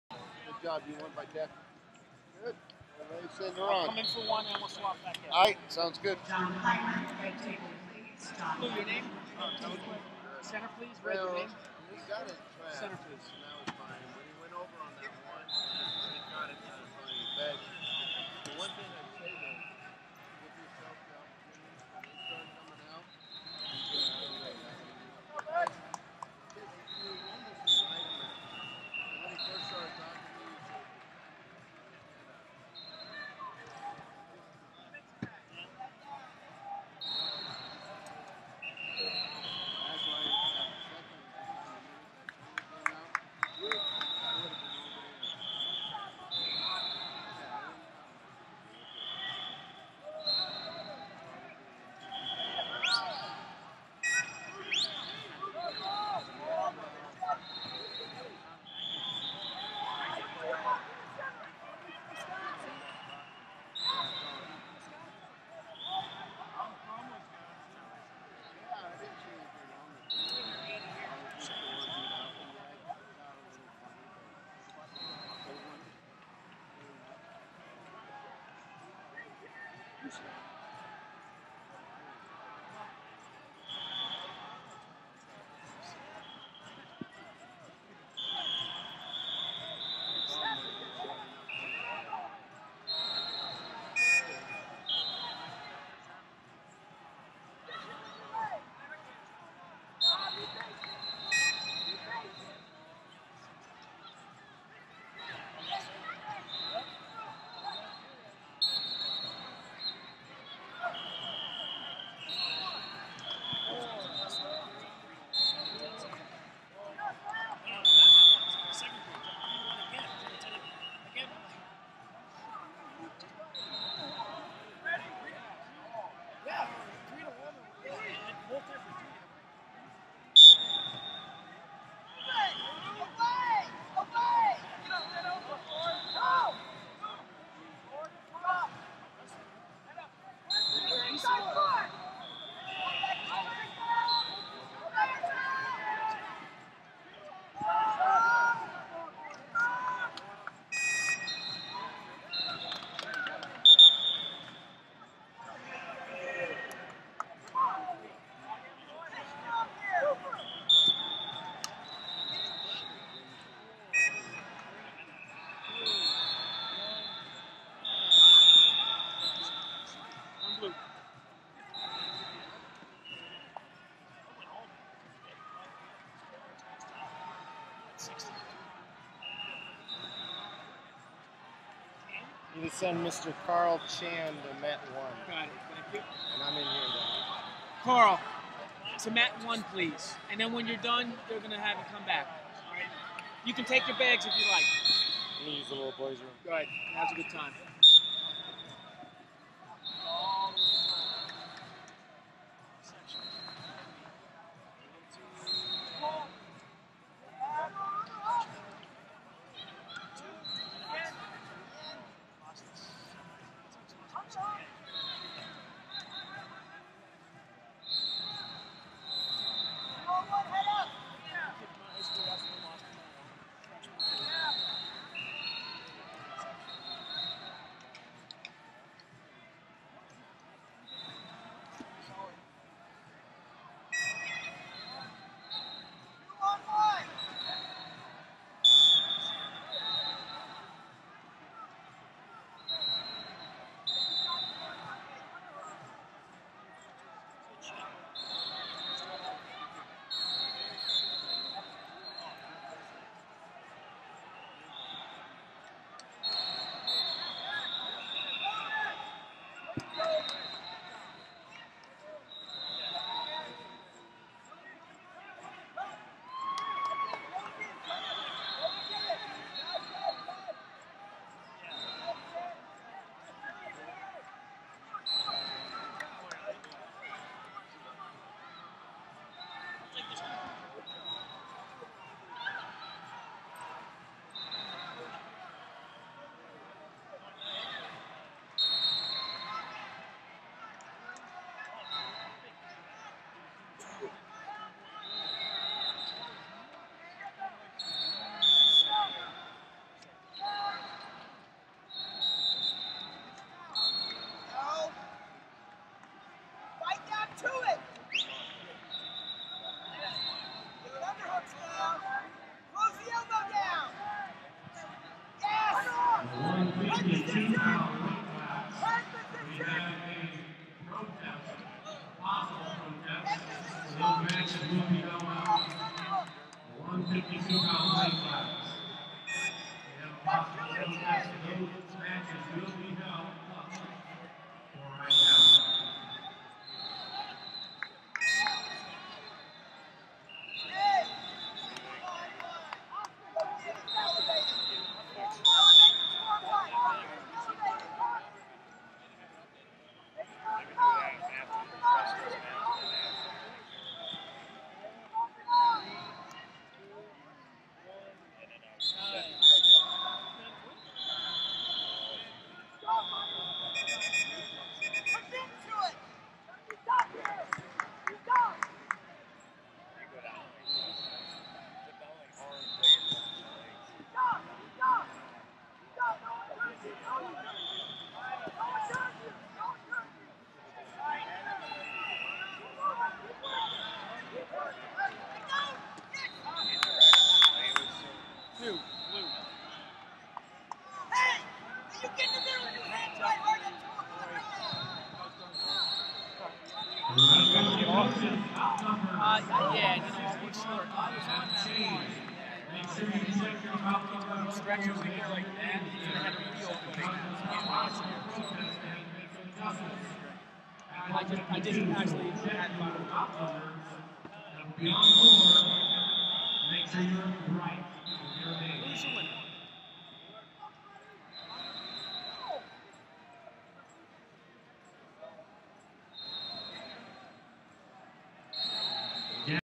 Fargo 2012 120 Round 6: Austin Lister (Oregon) vs. Joe Velliquette (Missouri)
Round 6 match of the 2012 USAW Cadet Greco Nationals with Austin Lister from Oregon vs Joe Velliquette from Missouri at 120 lbs.